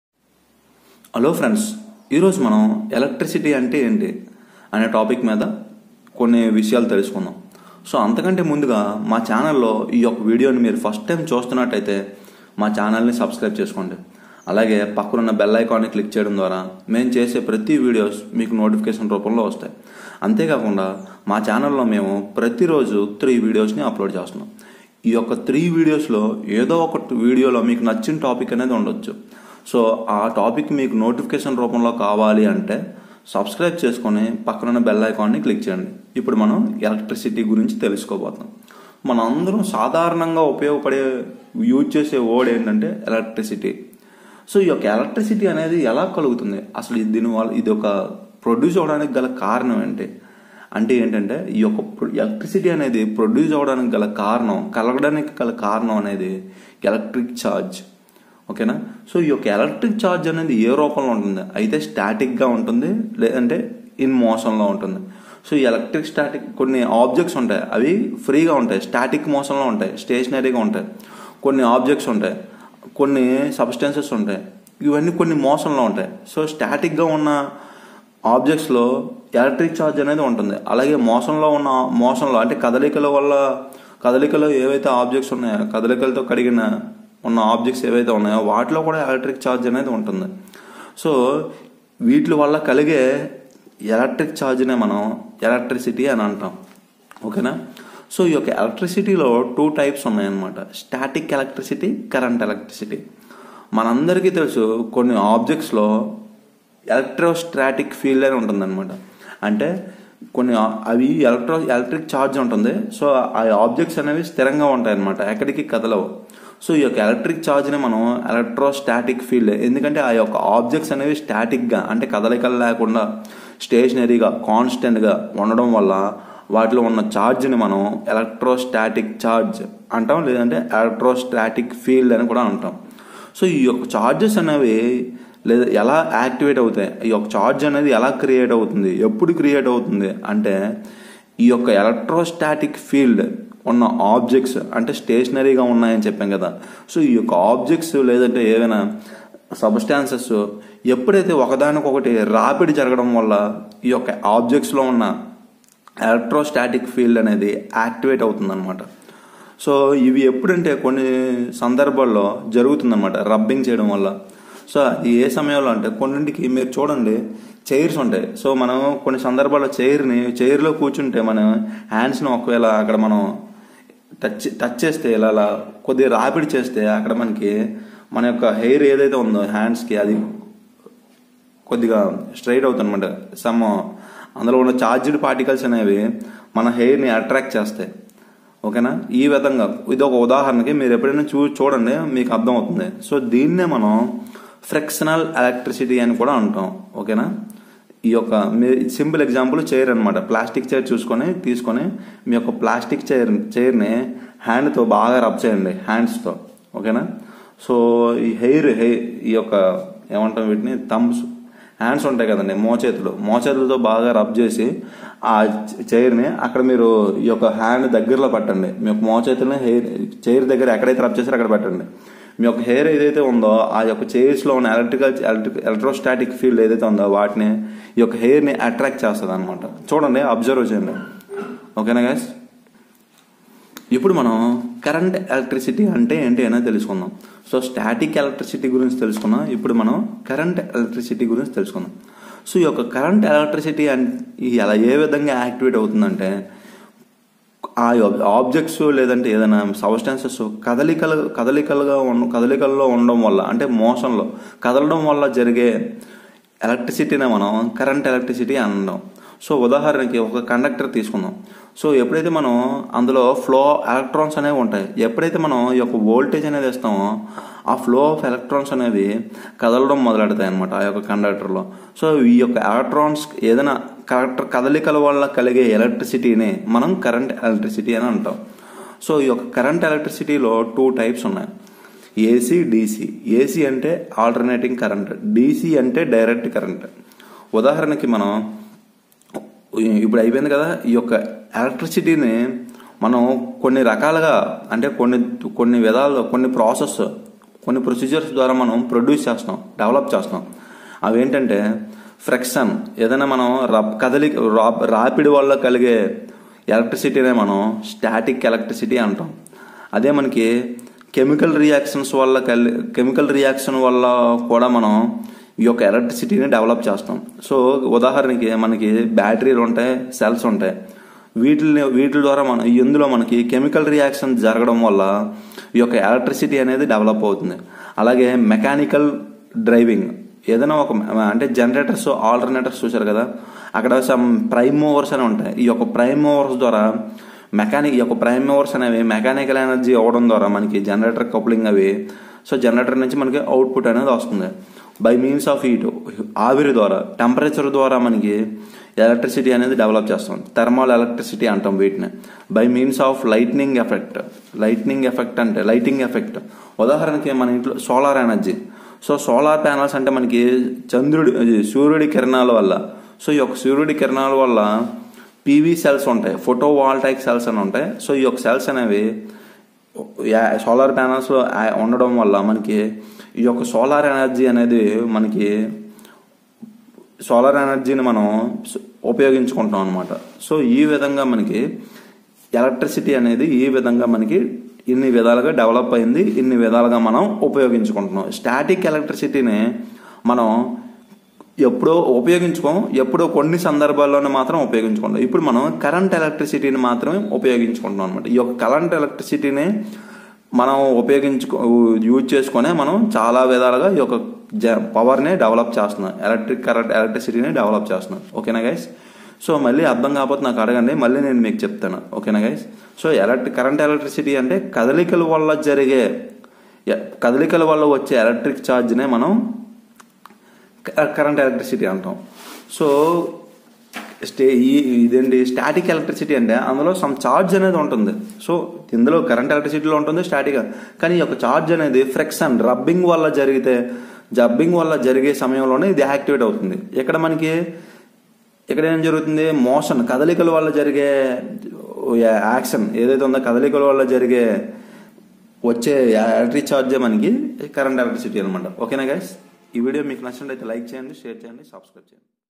What is electricity static electricity current electricity define definition explanation telugu 2020
Electricity is caused by electric charge  which is in static or motion form